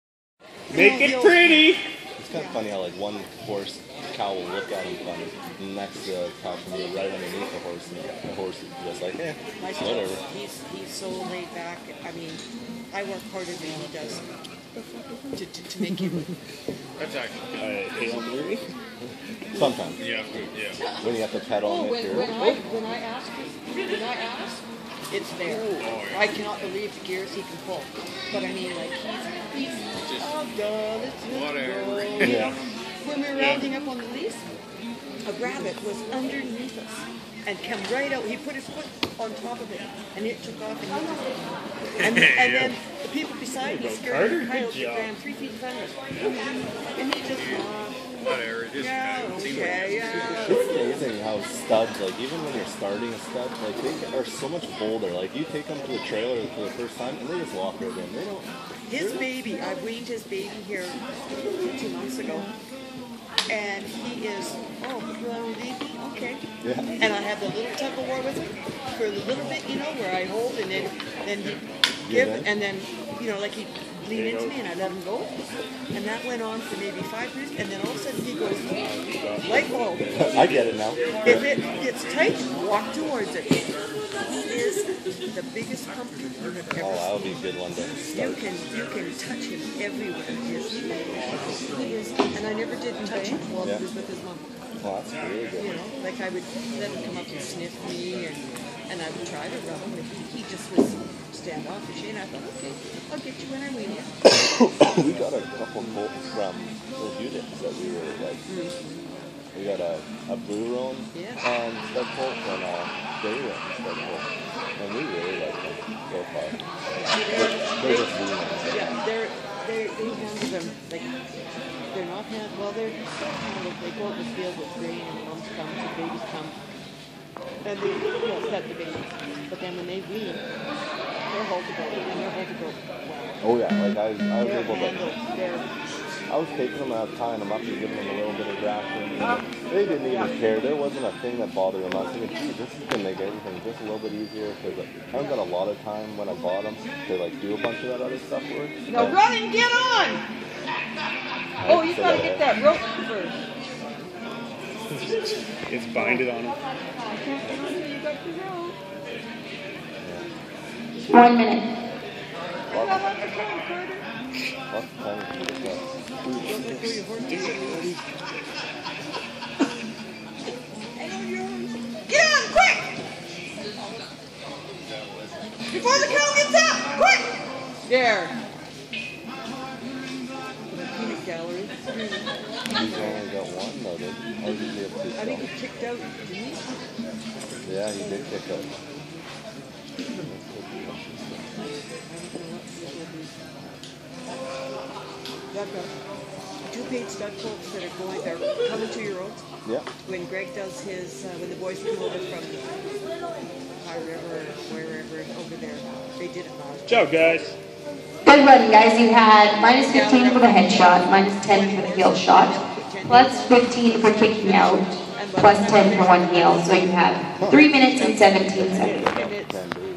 Carter Link and Jeffrey Stout Ranch Roping
Ranch roping competition @ Silver Sage Brooks, AB